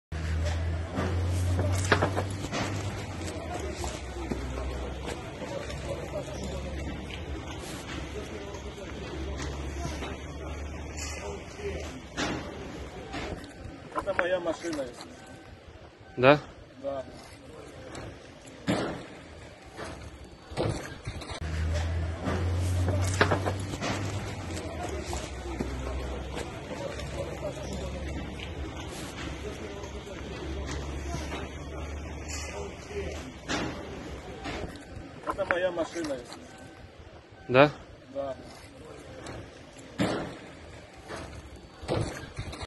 Firefighters Sift Through Rubble After Missile Strikes Kharkiv
Credit: @SESU_UA via Storyful